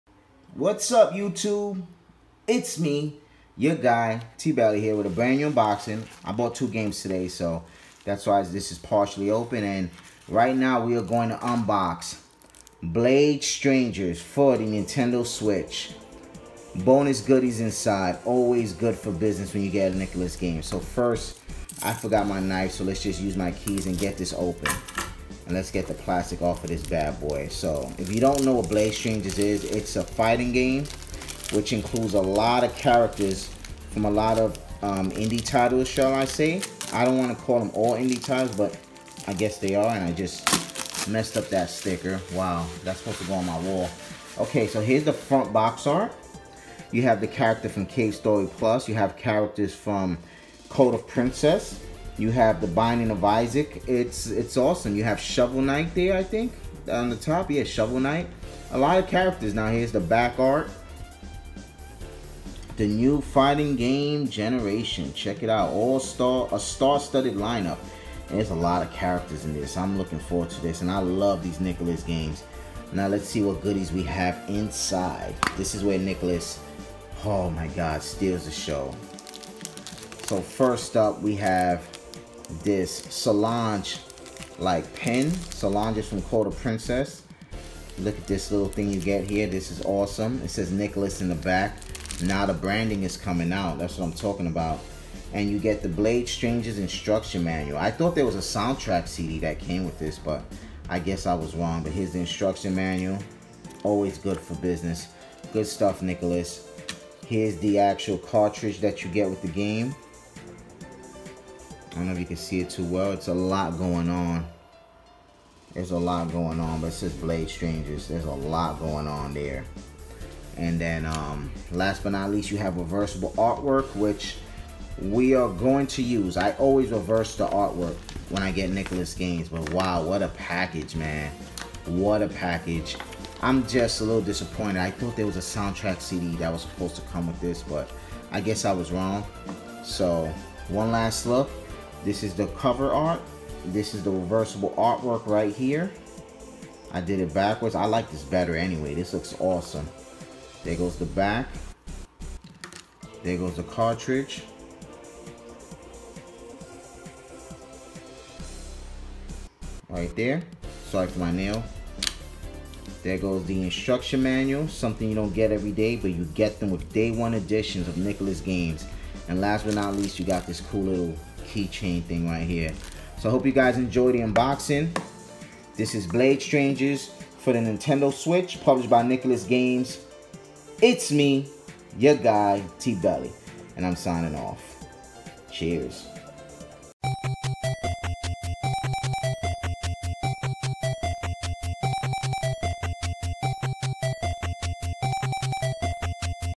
Blade Strangers Unboxing Switch
Quick Unboxing of Blade Strangers for the Nintendo Switch by Nicalis games. I apologize for not making this a 4K video, I forgot to change a setting.

Follow your guy TBelly on Social Media and online gaming

PSN
Tdot21st